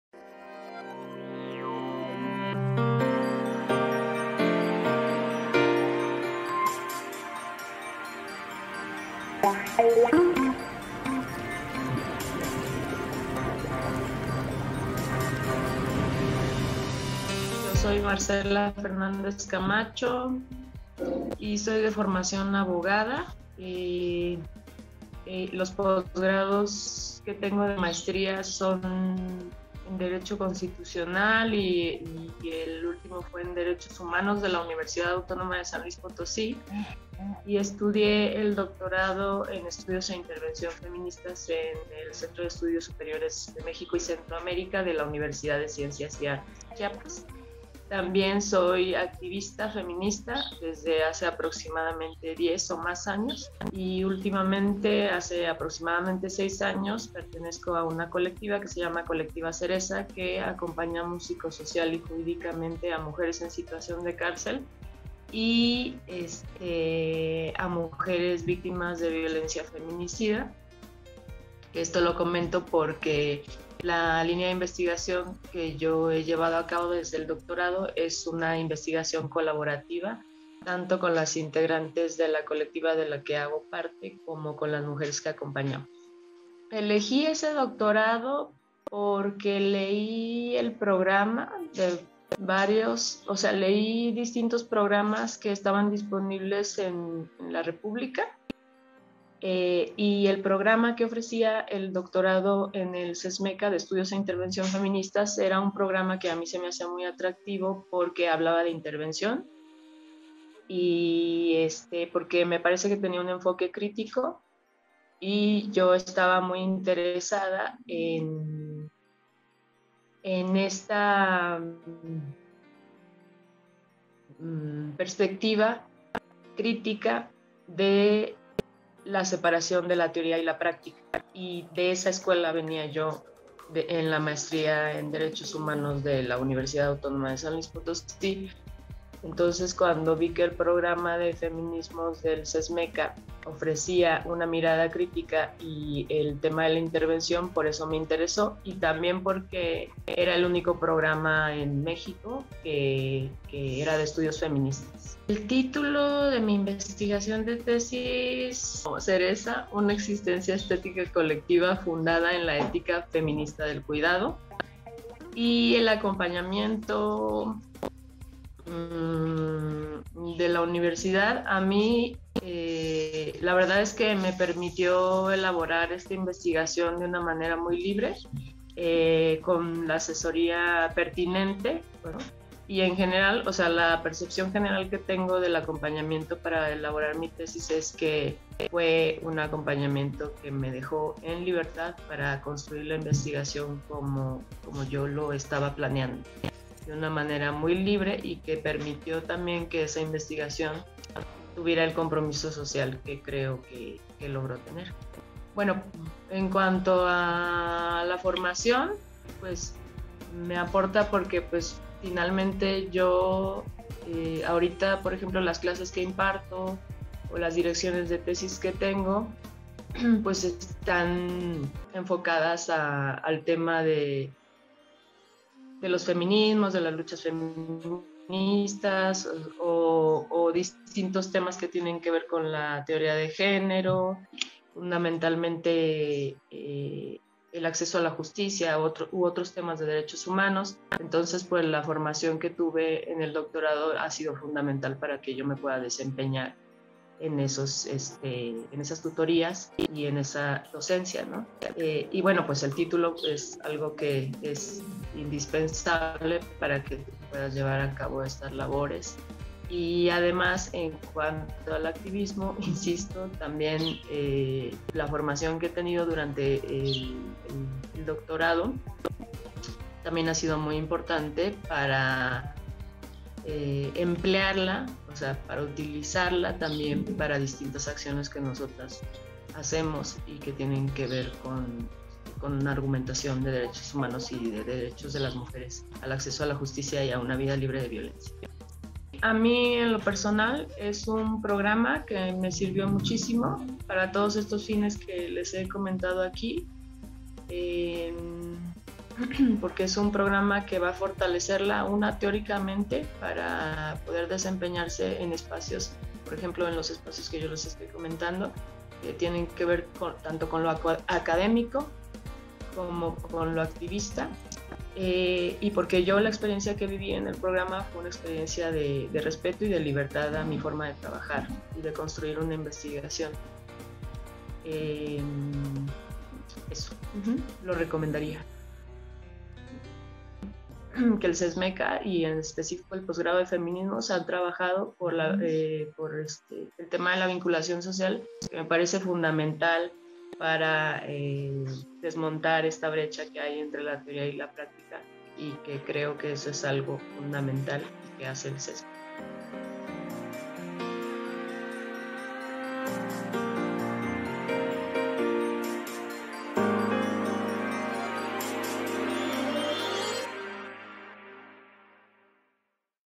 Marcela Fernández Camacho, egresada del Doctorado en Estudios e Intervención Feministas
Entrevista a Marcela Fernández Camacho, egresada del Doctorado en Estudios e Intervención Feministas, generación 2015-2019.

Convocatoria 2022: Maestría y Doctorado en Estudios e Intervención Feministas